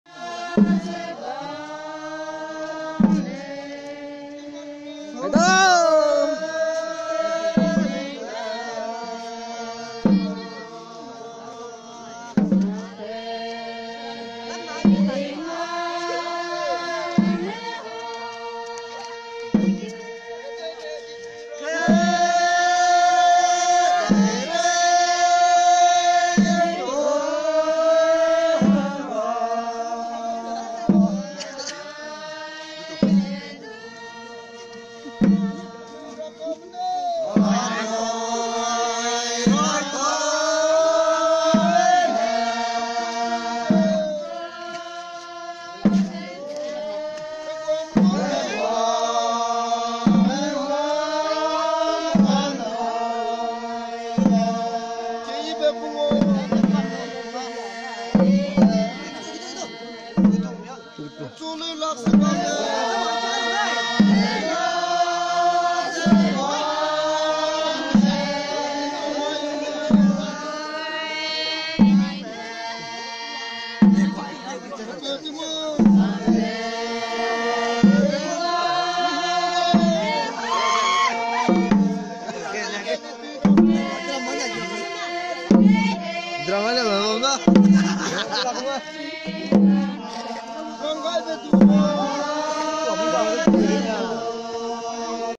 Zeme Traditional Happy Marriage Life at guilong😍😍
MARRIAGE...
It's a love marriage
from Guilong village...
Haflong/Assam/India.

Az.Rangtaukambe Nriame nai NL.Echubuile Nriame chu kahegia pei nchi gai delei. penai je Guilong kelo me karai kelak teu je bam delei.

KELUM...3 Thank You.